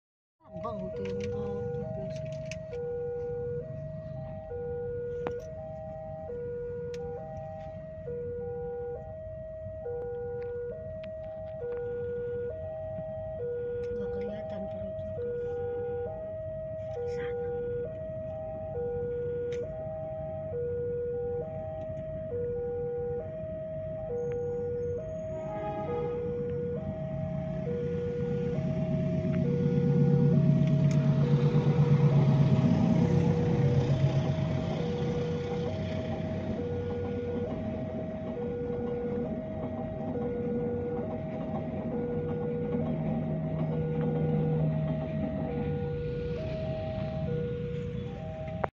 Hunting Lg Di Cirebon ygy,, KA Bogowonto FT CC 203 02 04 MN
Ciri Ciri KA NYA :

Bawa C3 02 04
Bawa 4 K3 Kemenhub
Bawa KM1 Kaca Pesawat
4 Bawa K1 (3 K1 Kaca Pesawat & 1 K1 Kaca Lebar)
Bawa Pembangkit

Itu KA APA ya ?, kok kek kenal ?

Tak remake, karna udh di kasih tau sama @MusaRailfansCirebon

thanks @MusaRailfansCirebon Udh Di kasih tau ka ini